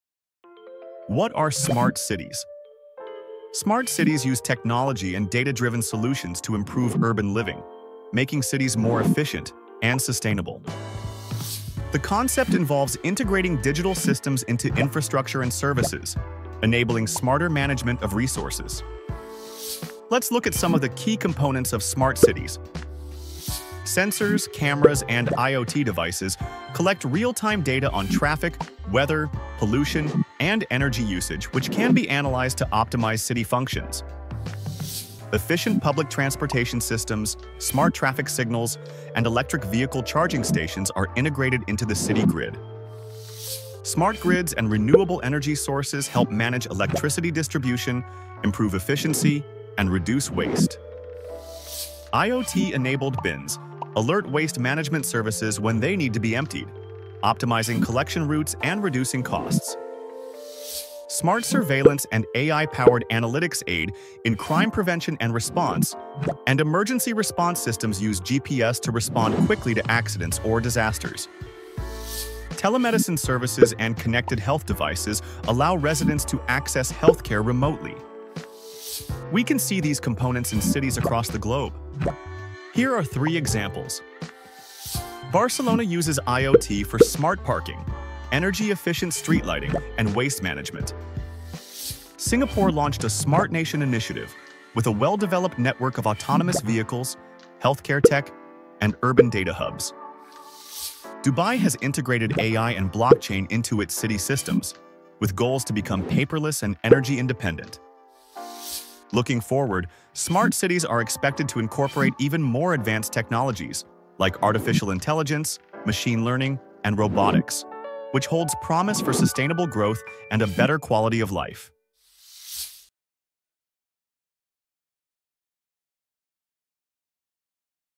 Smart Cities
Our video explains what Smart Cities are and how they transform urban living. SmartCities use cutting-edge tech to create cleaner, more connected, and sustainable communities. Dive into the future of urban life and see how Innovation makes cities smarter every day!